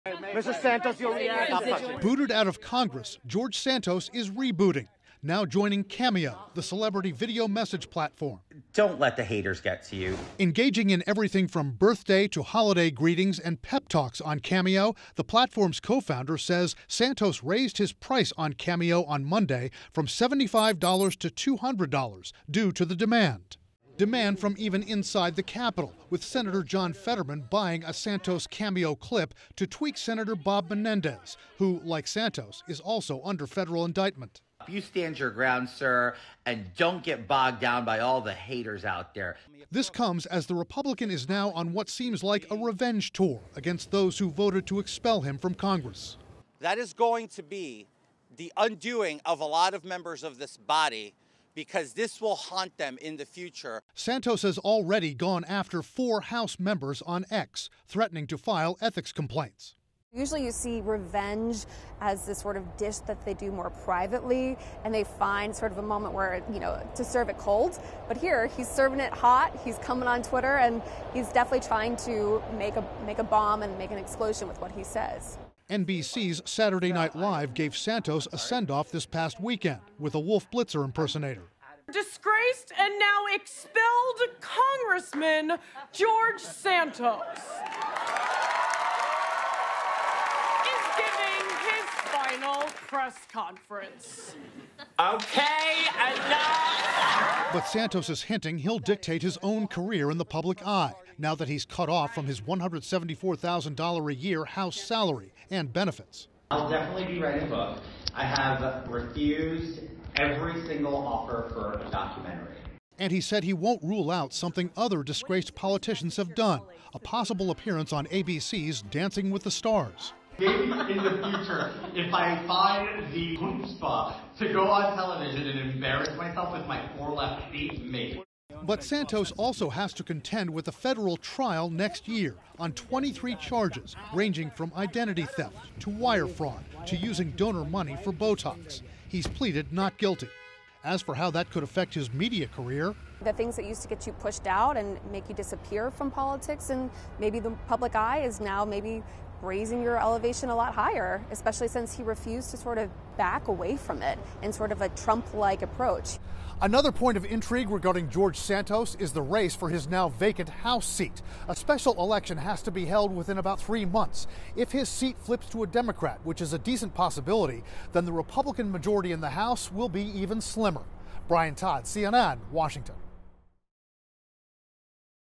See George Santos' surprise next move after being expelled from Congress
Former New York congressman George Santos is turning to the internet as his next move after his rare expulsion from Congress. CNN's Brian Todd reports.